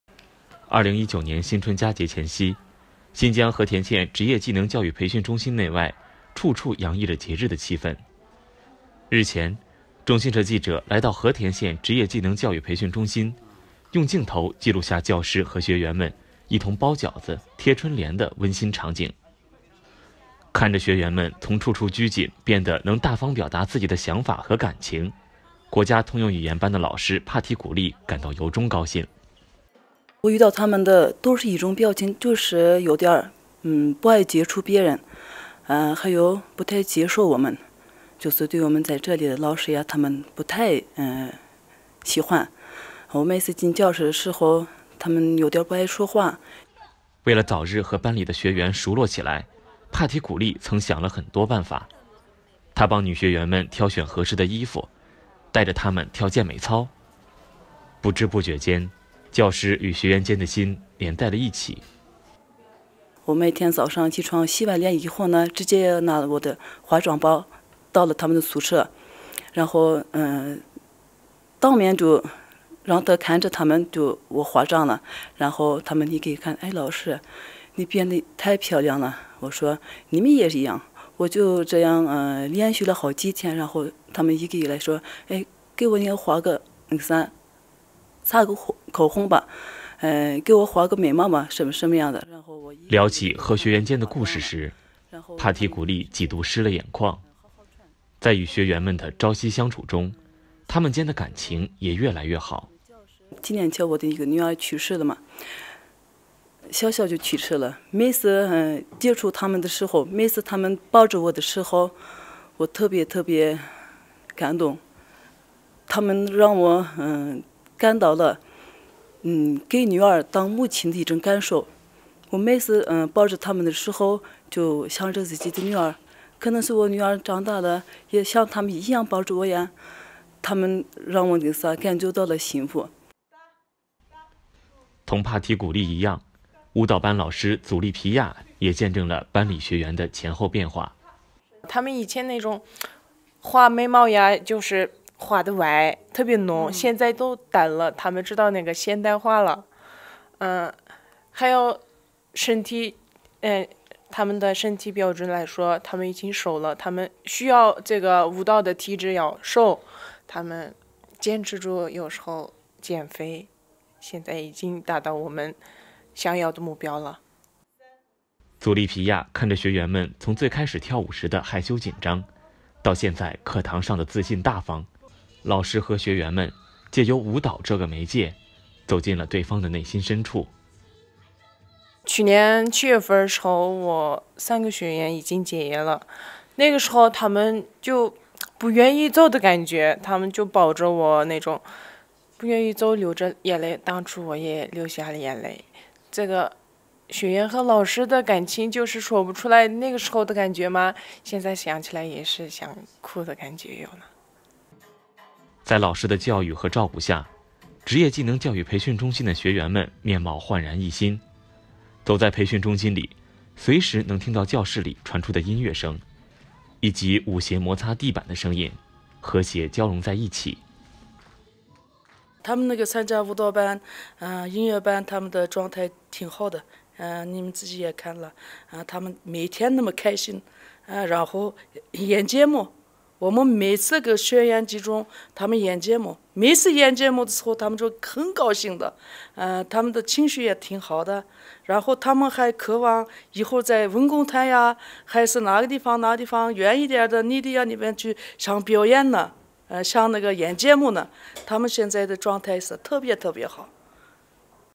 新疆教培中心教师：学员抱着我的时候让我想起女儿
2019年新春佳节前夕，新疆和田县职业技能教育培训中心内外，处处洋溢着节日的气氛。日前，中新社记者来到和田县职业技能教育培训中心，用镜头记录下教师和学员们一同包饺子、贴春联的温馨场景。